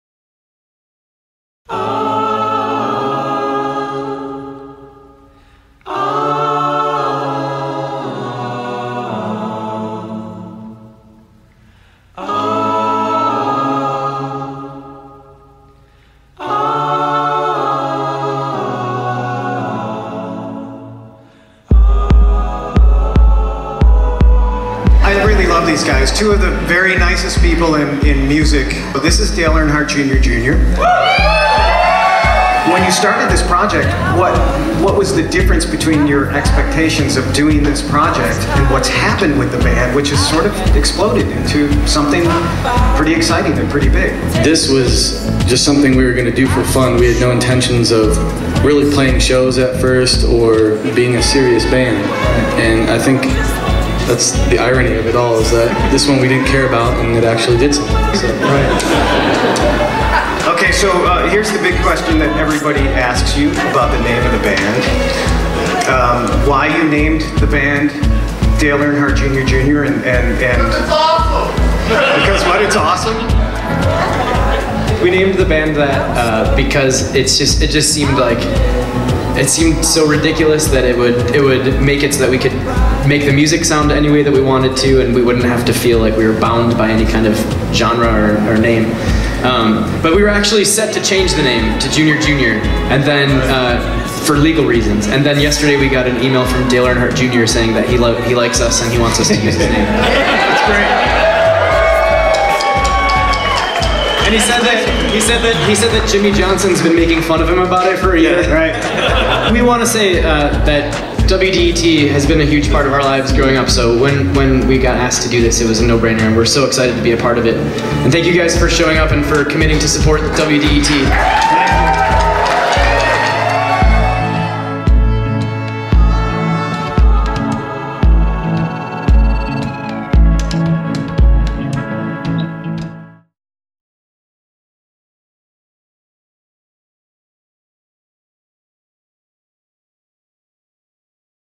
WDET - Jon Moshier: Dale Earnhardt Jr. Jr. @ The Crofoot
4/29/11
A Night of Modern Music: Hosted by Jon Moshier
Dale Earnhardt Jr. Jr.
WDET.ORG